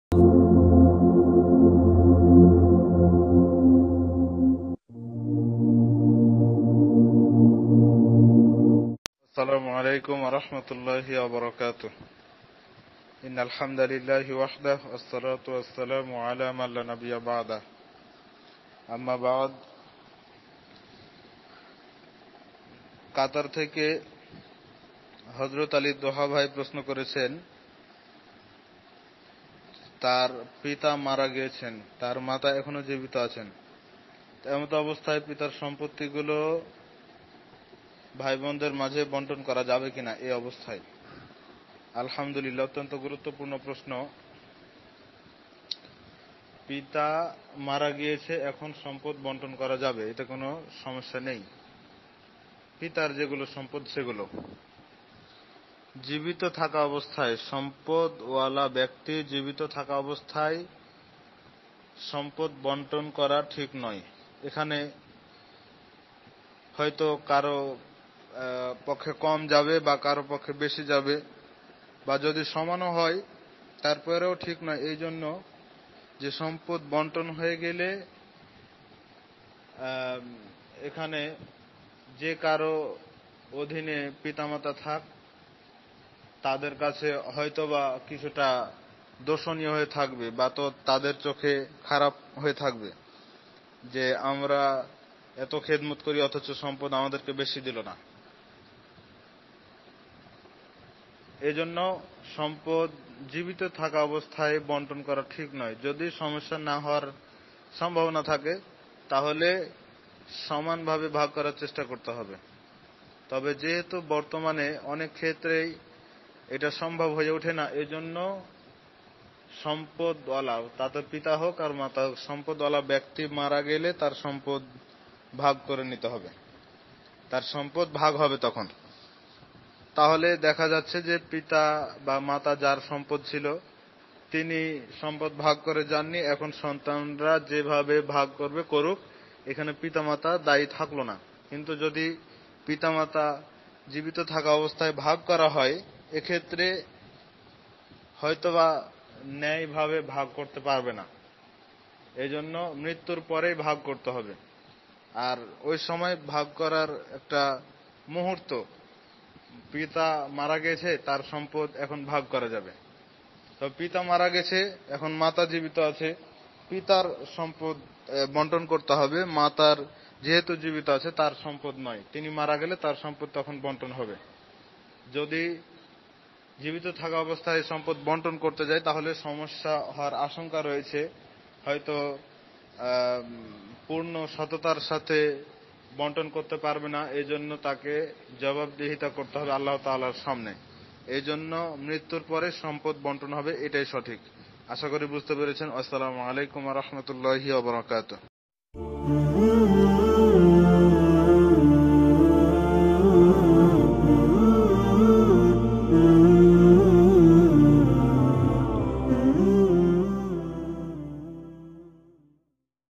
বাবার মৃত্যুর পর সন্তানের সর্বপ্রথম করনীয় কি? শায়খ আব্দুল মালেক বিন ইদ্রিস
বাবার মৃত্যুর পর সন্তানের সর্বপ্রথম করণীয় কি